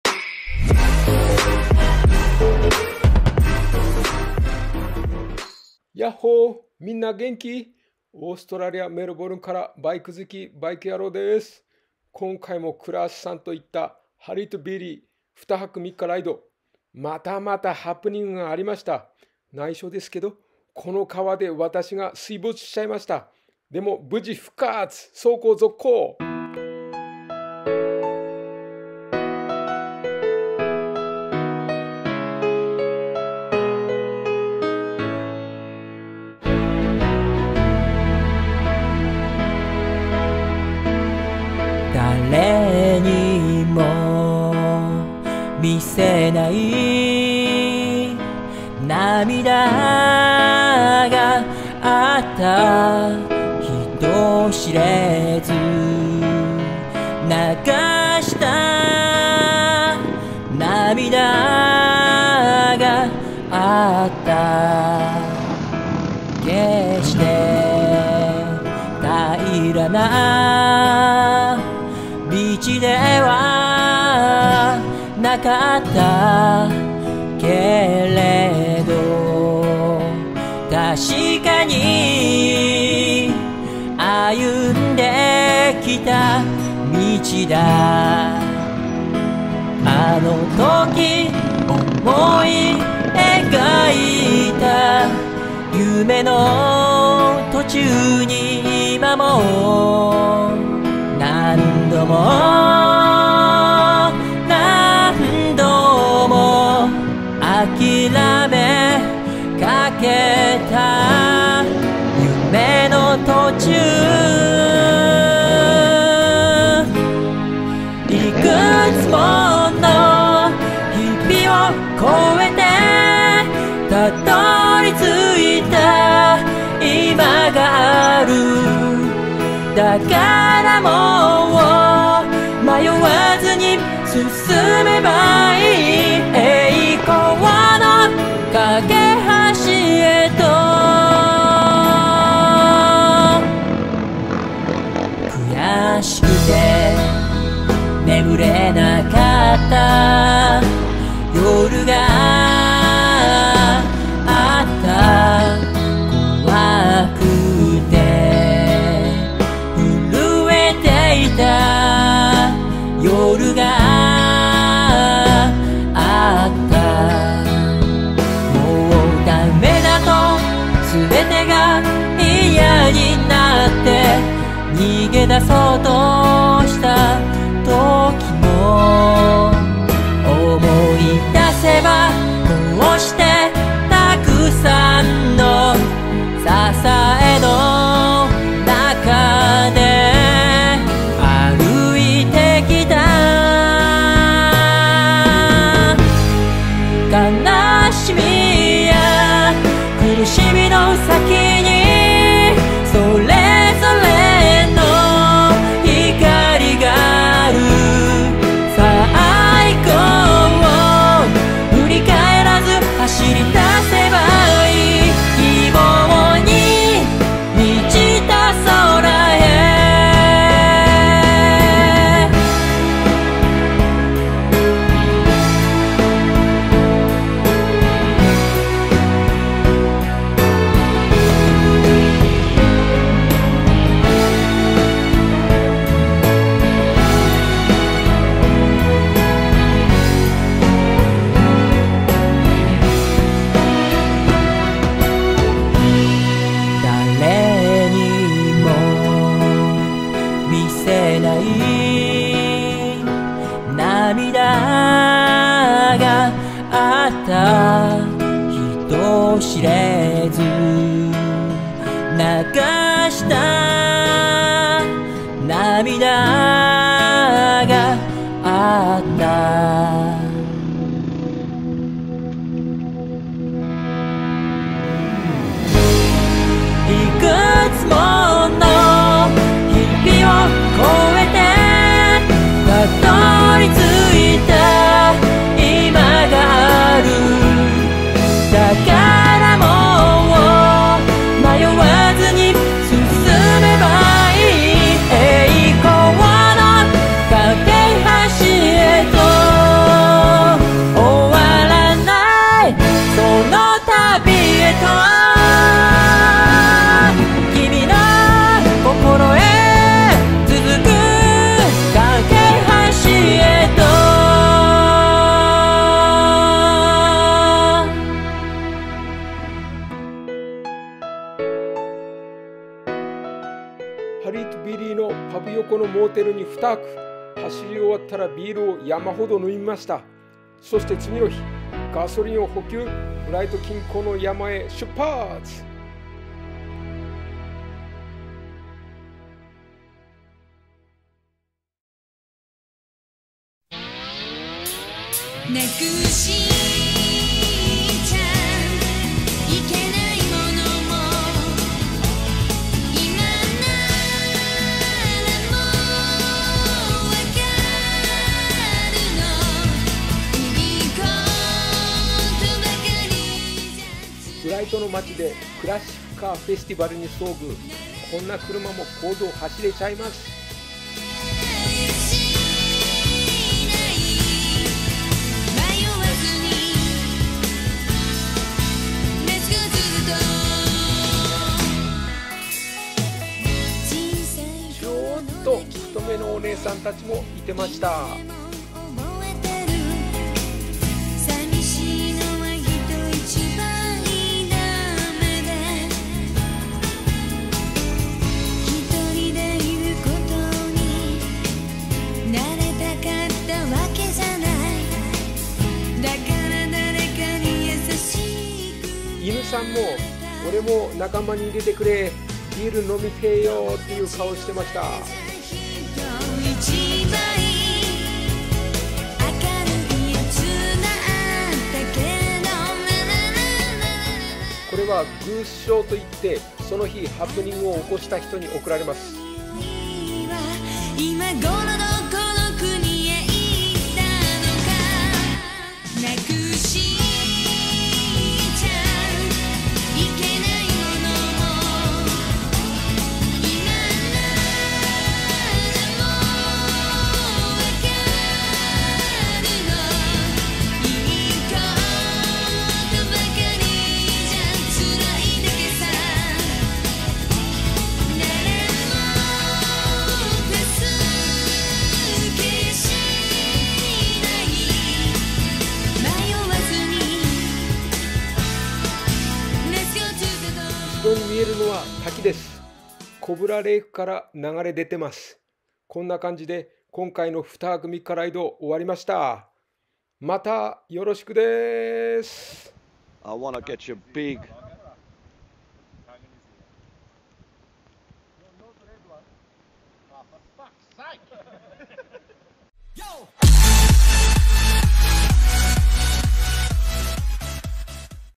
オーストラリアからバイク好きバイク野郎（内緒の水没）
仲間に水没後、どのように復活させるかを伝授！